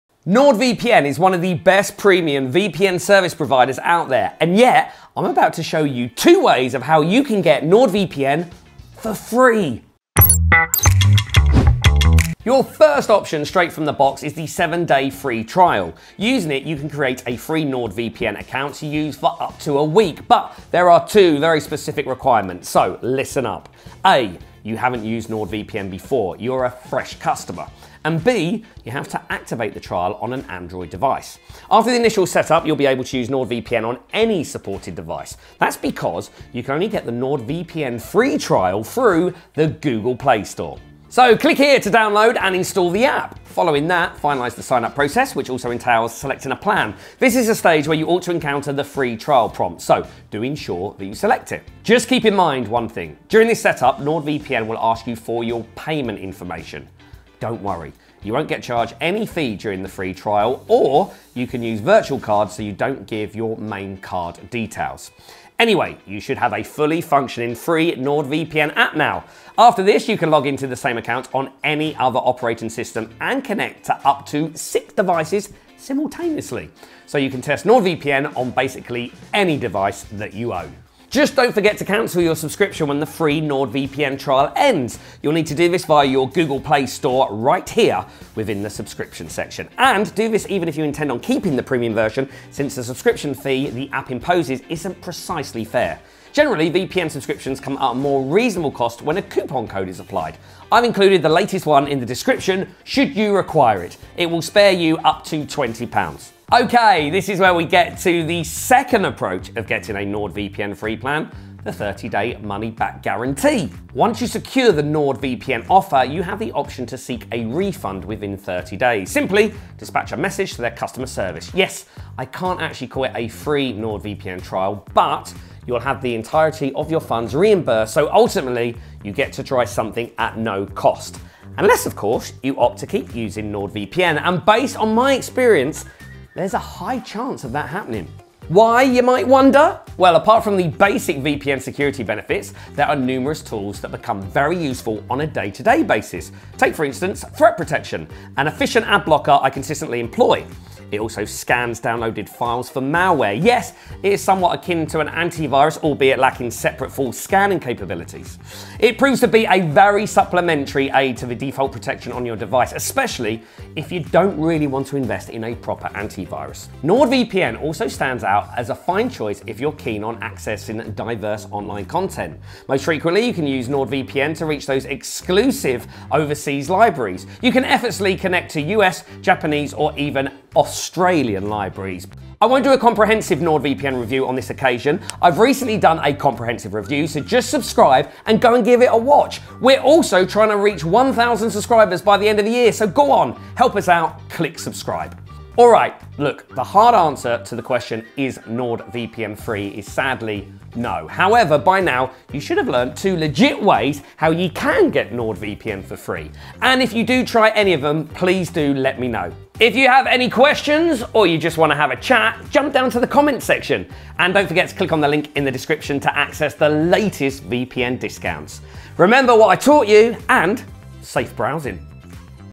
How to get NordVPN for Free | Easy Tutorial for 2024
🔥  Try NordVPN risk-free for 30 days: 🔥

NordVPN is one of the best premium VPN service providers out there. And yet, I’m about to show you 2 ways of how to get NordVPN for free.

Using this, you can create a free NordVPN account to use for a week. To start this, you need an Android specifically. Keep in mind that you’ll have to add payment details. Don’t worry though, it’s not gonna charge you till the very end of the trial.

You’ll have to add payment details during the signup process, that’s standard. But NordVPN IS NOT gonna charge you till the very end of the trial. Just don’t forget to cancel your subscription when the free NordVPN trial ends. You’ll need to do it through your Google Play store

🔑  The 30-day money-back guarantee

Once you get the NordVPN deal, you can request to get your money back within 30 days. All it takes is dropping a message to their customer service. It’s not exactly a free NordVPN trial, but you’ll get all of your money back, so in the end… you’ll spend nothing.

Final thoughts ➡️

Alright, look, the hard answer to the question "Is NordVPN free" is sadly NO. However, by now you should have learned 2 legit ways of how to get NordVPN for free. And if you do try any of them, please do let me know…

00:00 Intro
00:12 How to get NordVPN free trial
00:51 Does NordVPN charge during free trial?
01:21 Canceling the free trial (IMPORTANT!)
01:48 Getting an additional 30 days
02:10 Using NordVPN to the fullest
03:15 Conclusion